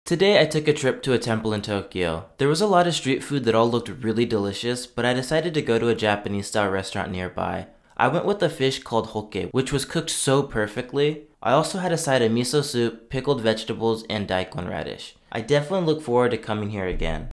This is What I Ate in Tokyo - Hokke (Fish)
Today, I took a trip to a temple in Tokyo and had to check out the food. I found a nice Japanese-style restaurant near the temple that was delicious. The name of this temple is Sensoji and it is located in Asakusa. Watch this video to find out more!